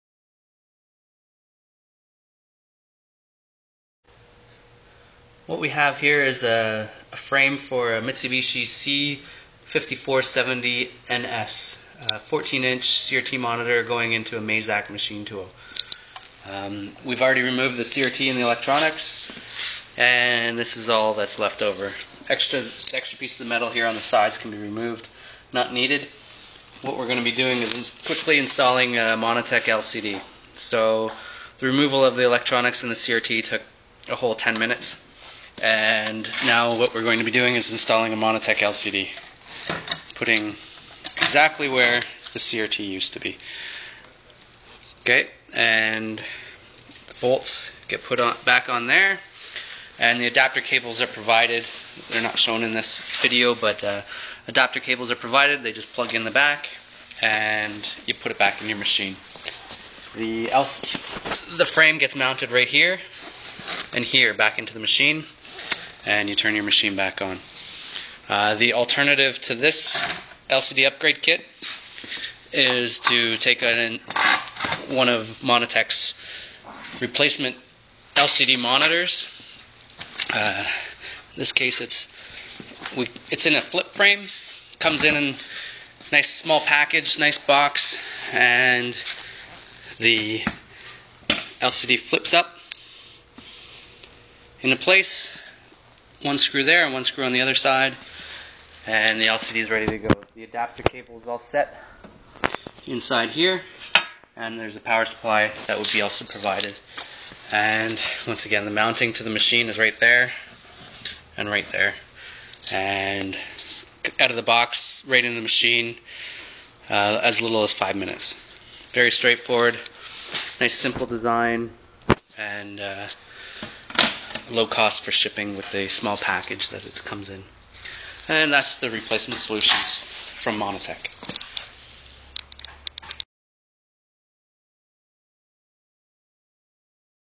C5470NS Mitsubishi Mazak LCD Upgrade from Monitech
This video walks through the LCD upgrade solution from Monitech for a Mazak 14" CRT Monitor.  C5470NS Mitsubishi

Video006